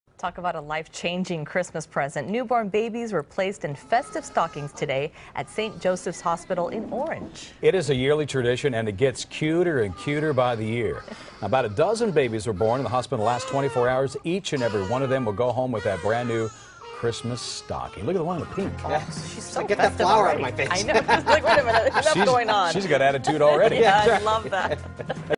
Newborns Placed In Festive Stocking Stuffers At Hospital In Orange
Newborn babies were placed in festive stocking stuffers today at St. Joseph Hospital of Orange. Jeff Vaughn and Suzie Suh report.